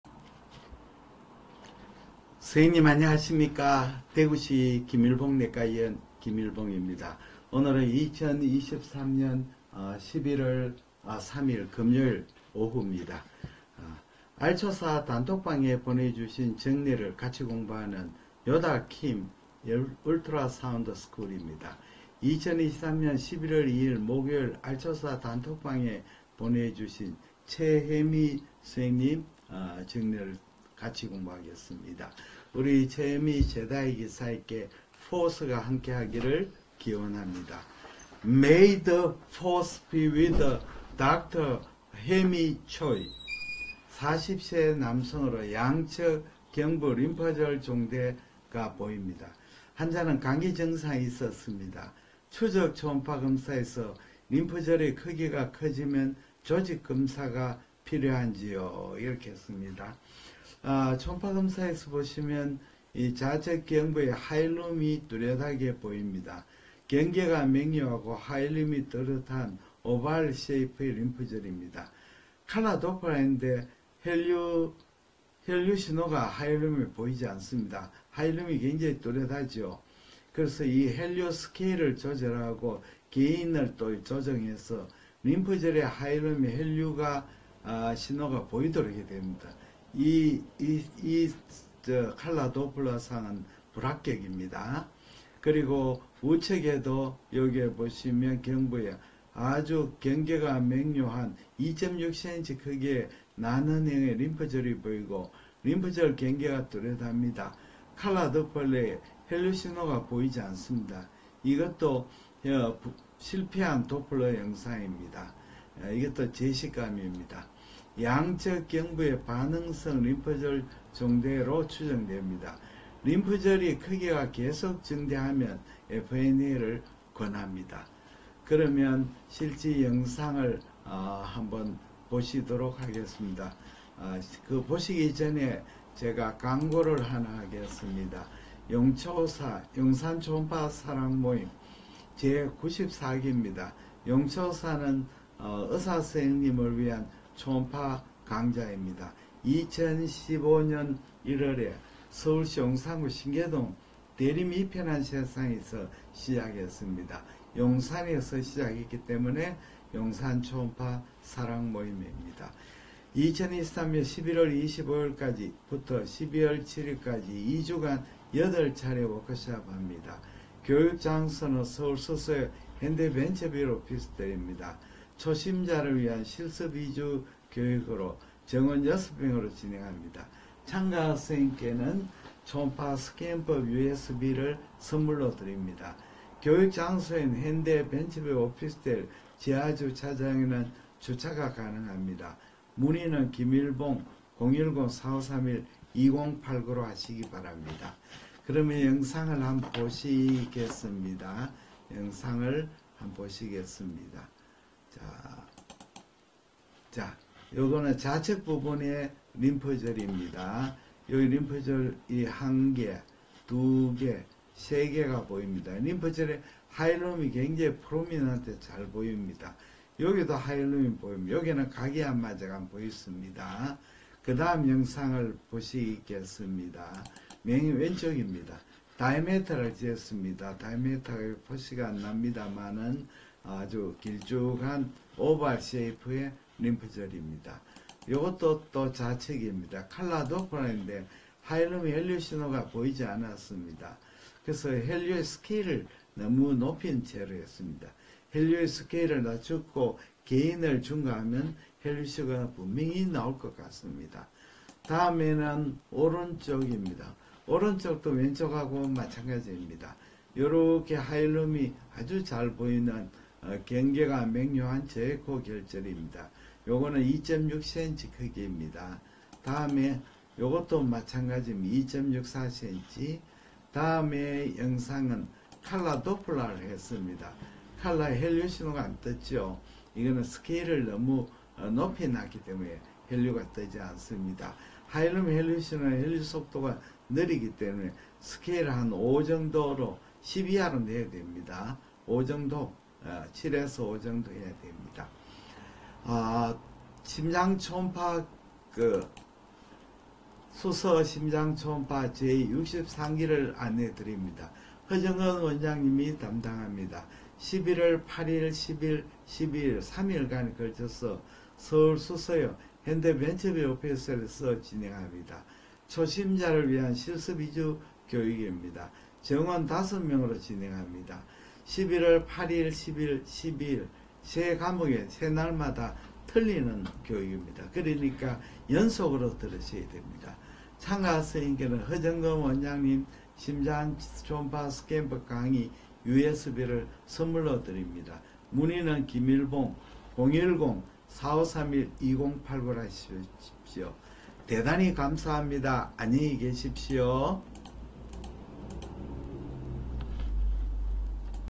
알초사 초음파 증례 최혜미 2023년11월2일 좌측 경부 다발성 반응성 림프절 종대 40세 남성
양측 경부에 다발성 경계 명료하고 길쭉한 형태, hilum 이 분명한 반응성 림프절 종대가 관찰됩니다.